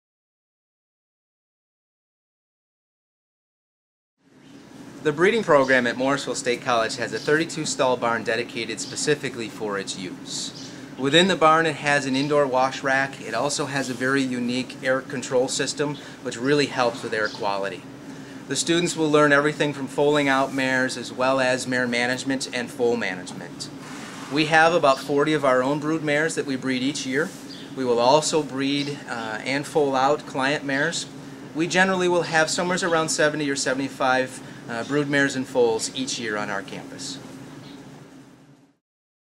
Equine - Breeding Barn
The breeding program at Morrisville State College has a 32-stall barn dedicated for its use. Students learn how to foal out mares and also learn about mare and foal management. Morrisville State College has approximately 40 brood mares that are bred each year, and students have the opportunity to breed and foal out client mares within this facility. There are approximately 75 brood mares and foals on campus each year.

The breeding barn is equipped with indoor wash racks and a unique ventilation system that controls and improves air quality.